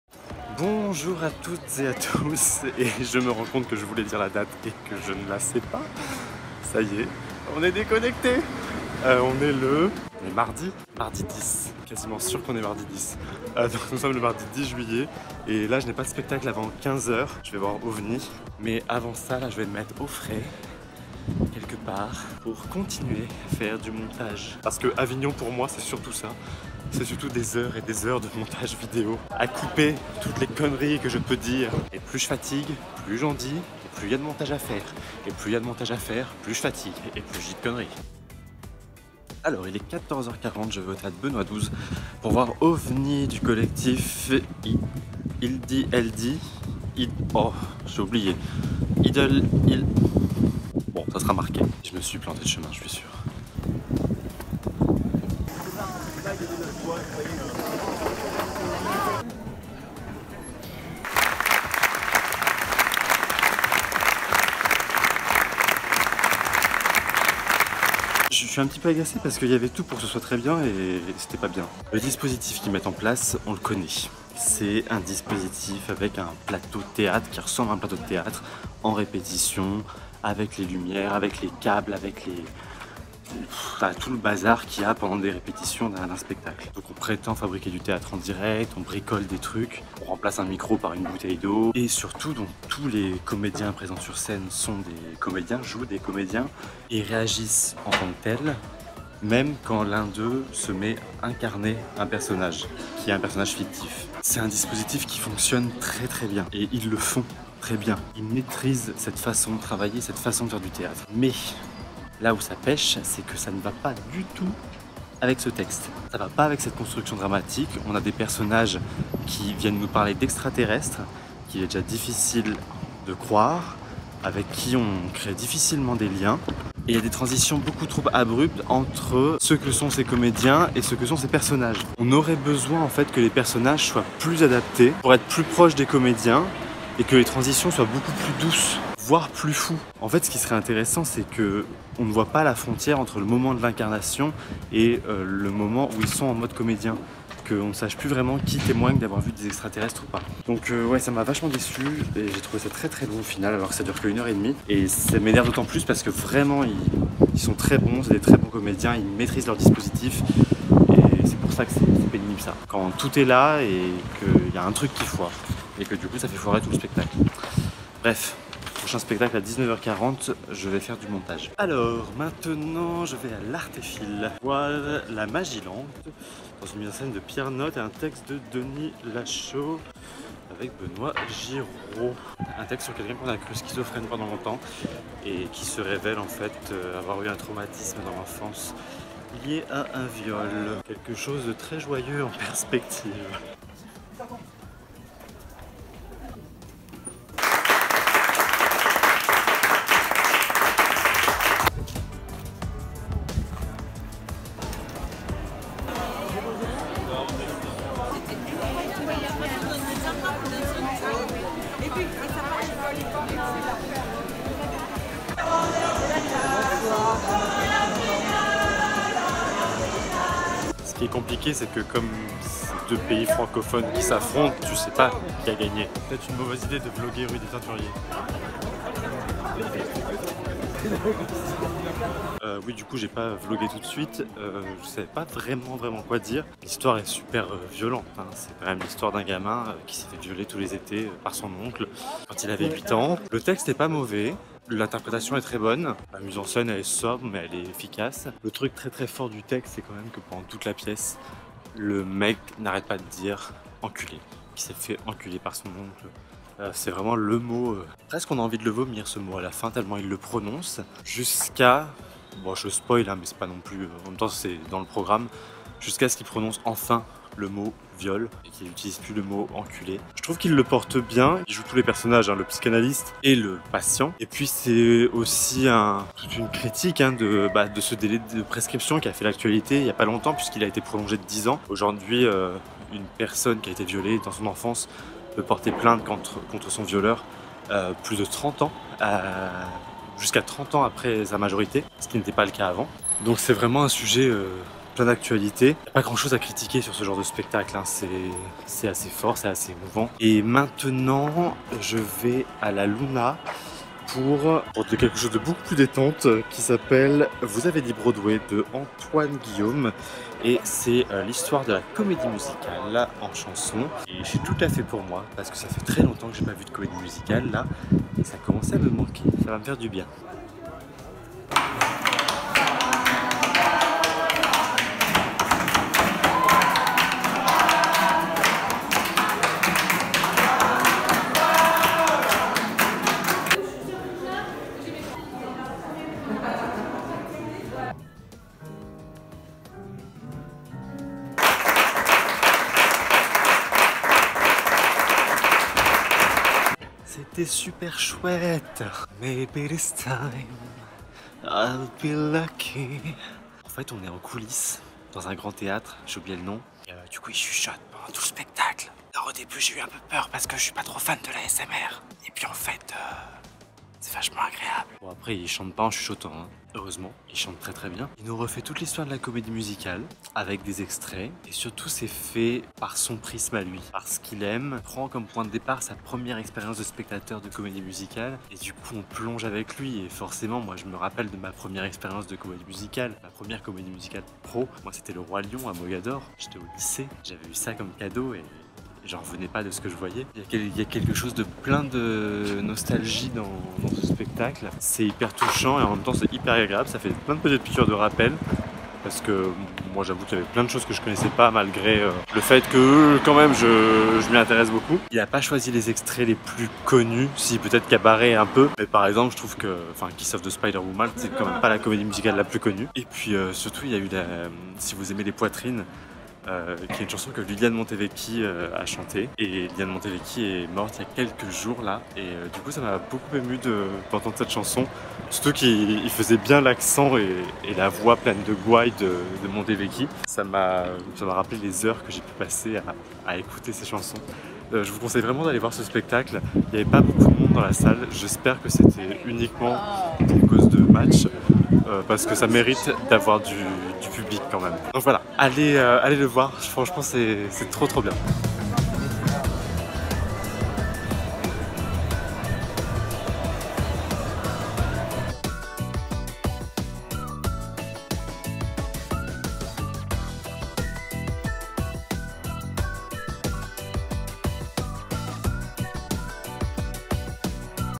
[VLOG AVIGNON #4] CHAMPION DU MONDE PENDANT LE FESTIVAL D'AVIGNON | RONAN AU THÉÂTRE
Au programme de ce 4ème Vlog au Festival d’Avignon :
Ovni(s) par le collectif Ildi ! Eldi au théâtre Benoit XII
La Magie Lente de Denis Lachaud, mis en scène par Pierre Notte, avec Benoit Giros à l’Artéphile
Vous avez dit Broadway, à la Luna, par Antoine Guillaume, hommage aux plus grandes comédies musicales de l’histoire.
Et évidemment du foot, même dans les rues d’Avignon, surtout lorsque les deux pays qui s’affrontent sont autant représentés dans la programmation du OFF.

RONAN est une chaîne youtube consacrée au Théâtre et d’une manière plus générale au spectacle vivant… Je vous emmène avec moi !